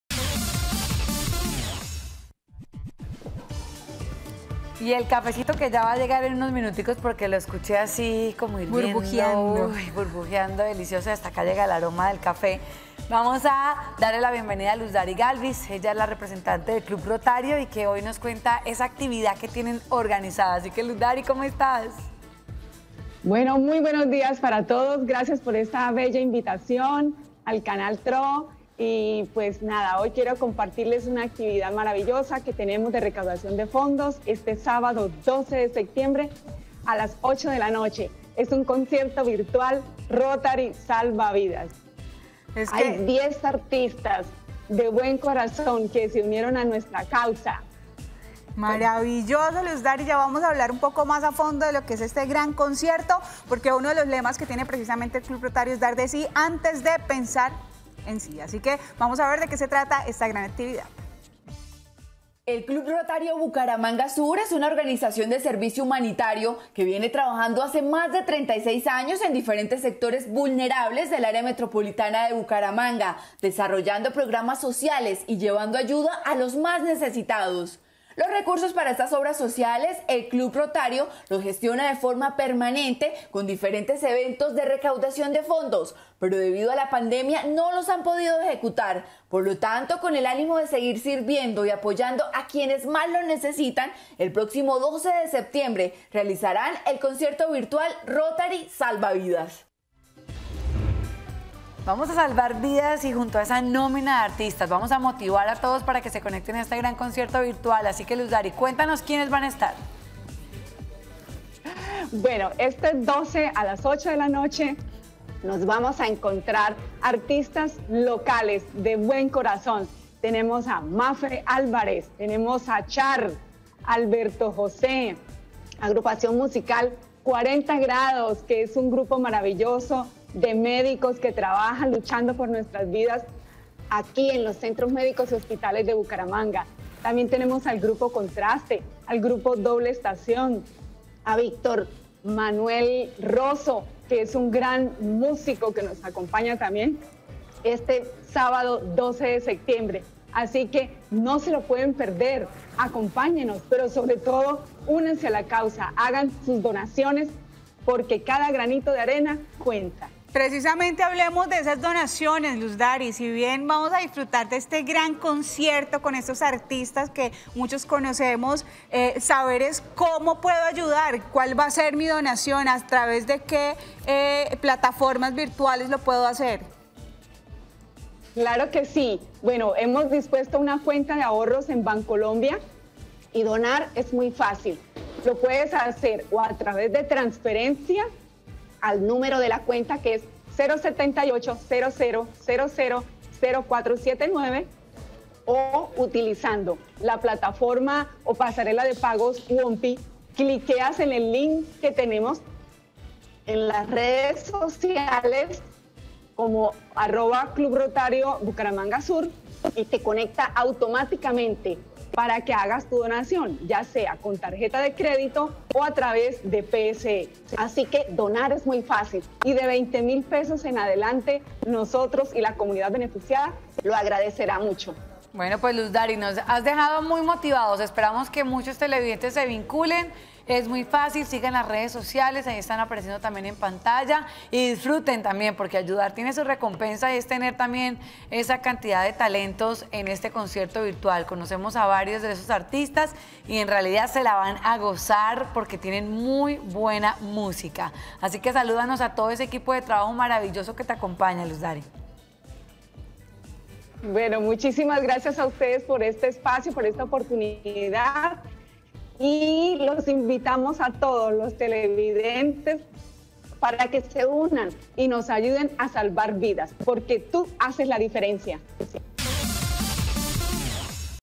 Concierto virtual 'Rotary Salva Vidas' en Santander
Hoy la presidenta del Club Rotario Bucaramanga Sur, Luz Dary Galvis, nos acompañó para contarnos acerca del concierto virtual ‘Rotary Salva Vidas’ que se desarrollará este sábado a partir de las ocho de la noche.🤩

Acompáñanos a ver la nota a continuación, conoce qué artistas apoyarán la causa y cómo tú puedes aportar el granito de arena.❤️